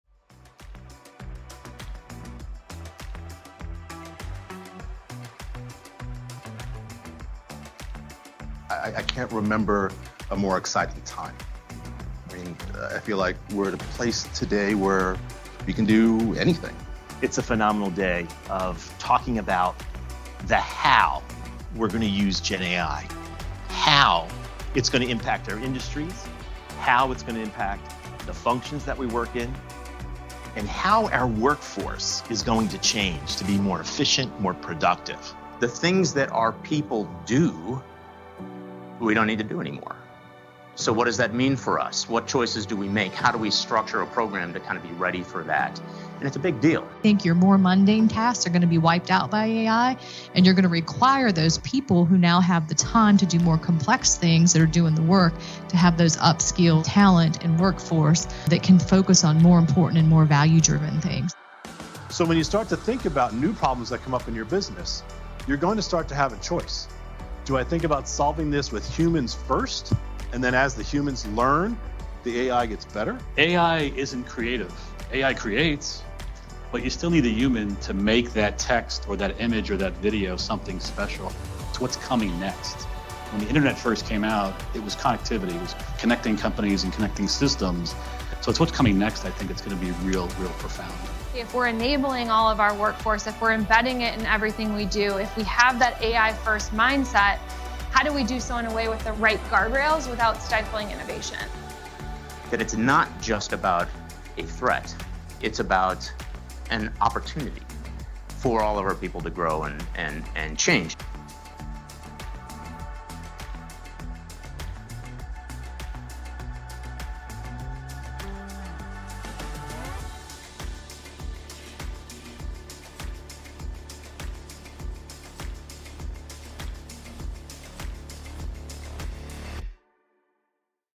KPMG Boston hosts first GenAI Executive Accelerator event
On May 8th, KPMG Boston brought together more than 150 executive and thought leaders to examine how GenAI will impact our businesses and industries, and to discuss practical considerations for creating value and investing in the right GenAI programs. Watch this video for highlights from the day.